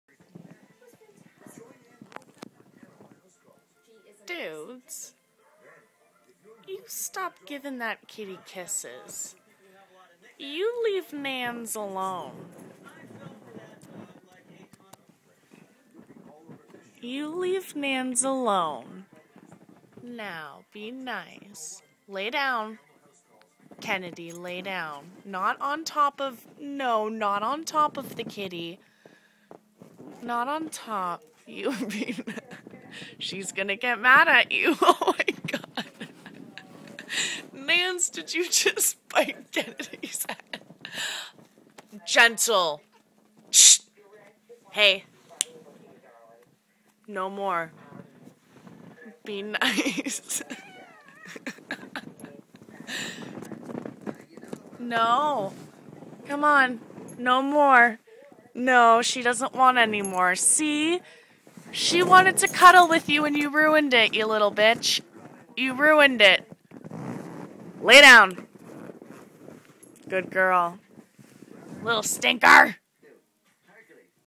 iPhone Moments with Lesbian Nicole #1- CUTEST Mortal Combat Ever (Dog VS Cat)
Kennedi gets the wrath of a bitey Annika, but over all wins this duel.